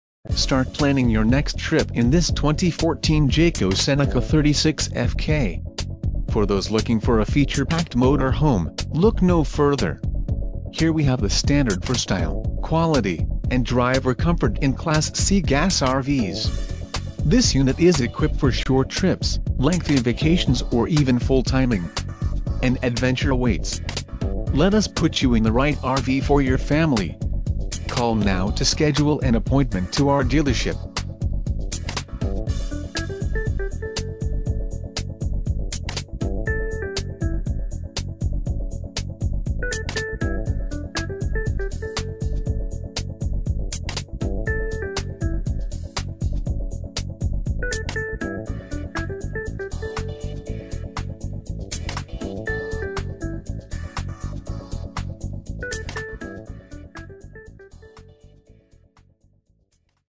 2014 Jayco Seneca - Colerain RV - OH, IN 45251
Plan for the trip of a lifetime with this grand 2014 Jayco Seneca 36FK. Featuring 36 feet of length, this Class C diesel motor home offers huge double slide outs and comfortable sleeping arrangements for6 guests! , Customer Value Package:\r\n12 cu.ft refrigerator w/ ice maker,\r\n(2) 15,000 BTU A/C w/ heat pump,\r\n1800 Watt invertor,\r\nBack-up and side camera and monitor,\r\nConvection microwave,\r\nElectric awning w/ LED lights,\r\nPower electric cord reel,\r\nIn motion minimax satellite dish,\r\nTouch screen in dash sound system w/ built in GPS,\r\nCD/DVD w/ MP3 input jack,\r\nDeluxe power driver and passenger seats,\r\nTankless water heater,